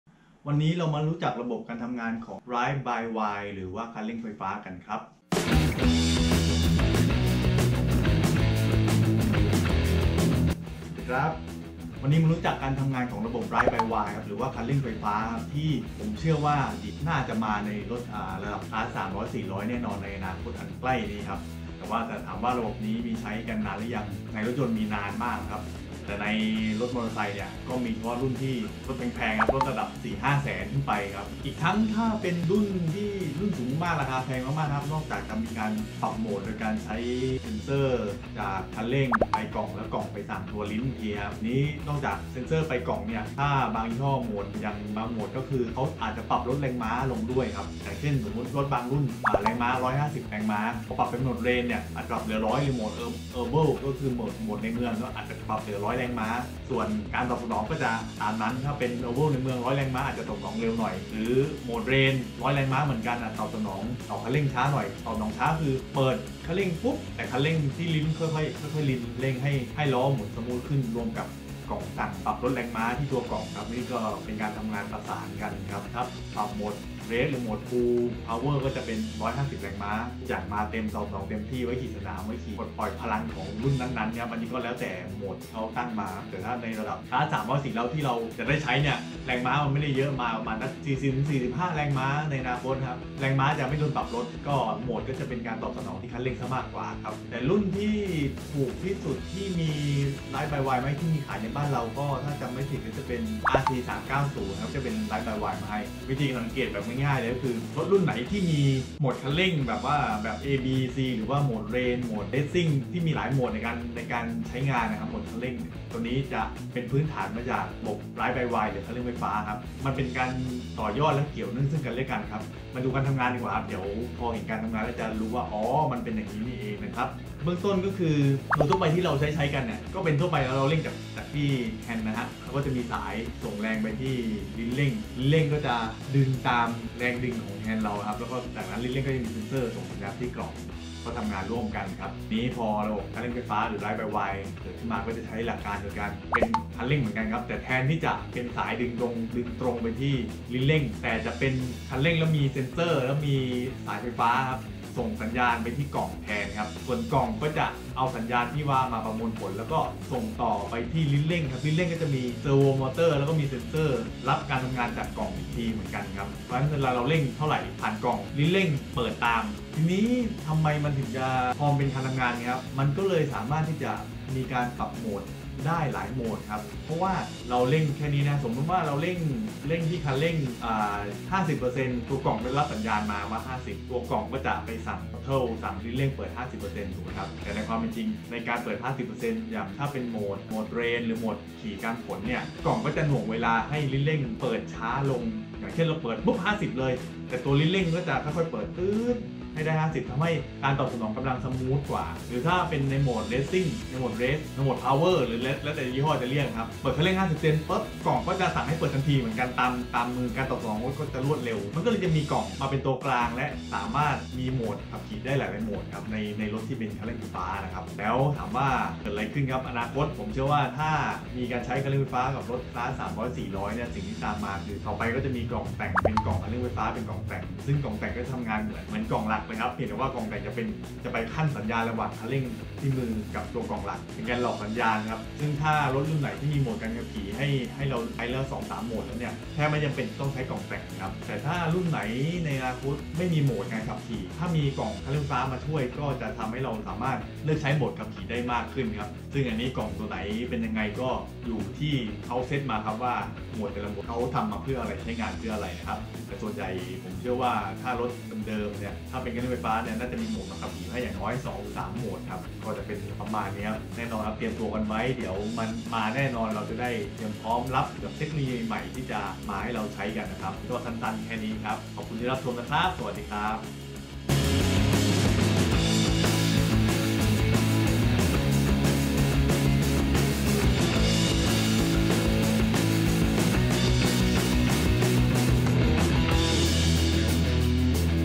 โม้เรื่องรถ ep.46 : คันเร่งไฟฟ้า หรือ ride by wire ทำงานยังไง
คันเร่งไฟฟ้า หรือ ride by wire อีกไม่นานเราคงจะได้ใช้กันในรถคลาส 300-400cc ครับ ตอนนี้มารู้จักมันไว้กันดีกว่า